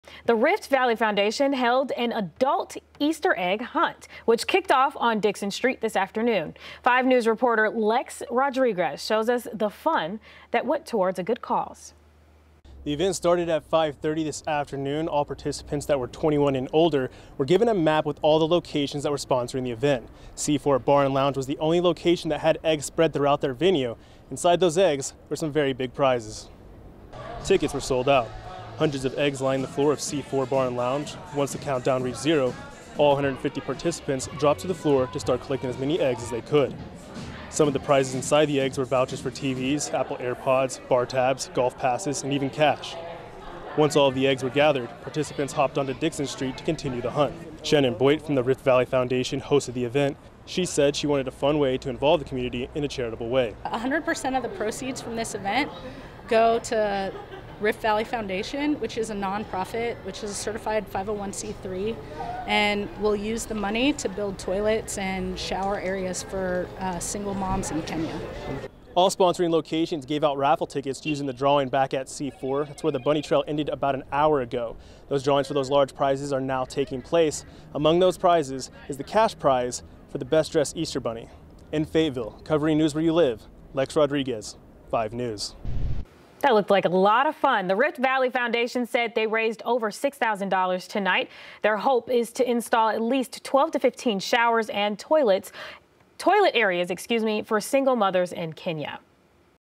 Adult Easter egg hunt takes place in Faytteville
The Rift Valley Foundation hosted an adult Easter egg hunt in Fayetteville, Saturday, April 16.